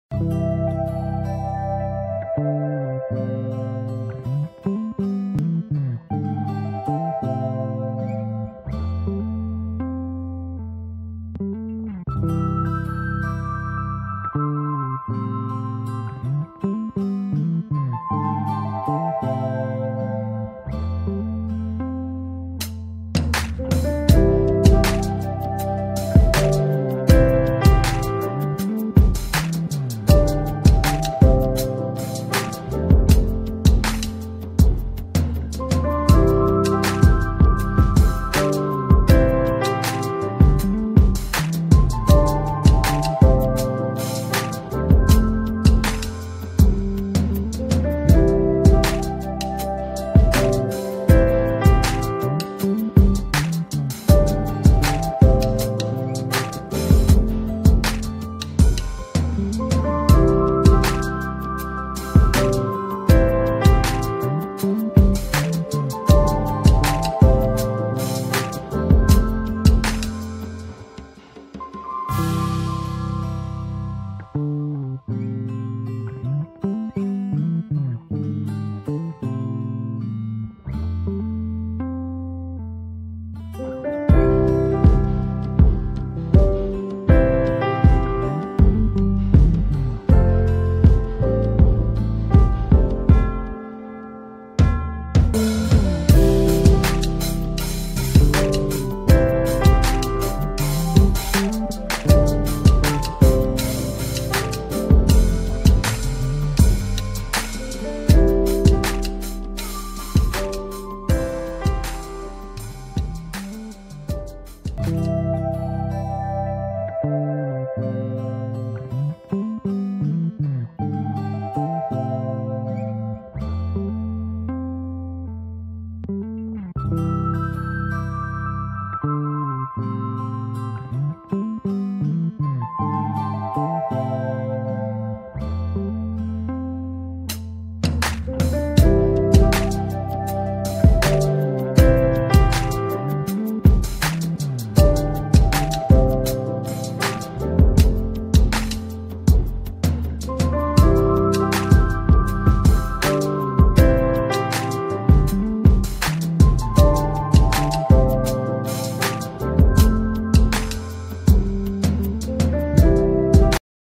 Goan fresh prawn curry with lady finger in earthern vessel on chulha |Goan traditional recipe
shrimp curry goan style
Goan traditional cooking
prawn curry recipe goan style